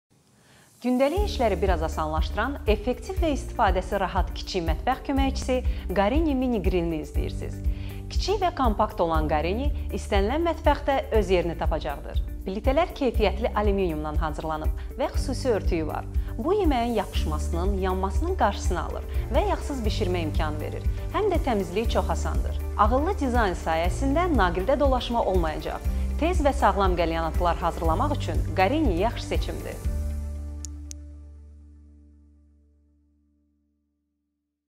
İzqara - Gorenje SM701GCB
İzqara ilə artıq peşəkar aşpaza çevrilə bilərsən! Əziyyətsiz və rahat qəlyanaltı hazırlamaq istəyənlər bu məhsulu çox sevəcək. Müxtəlif mətbəxlərə məxsus təamları öz mətbəxində hazırla!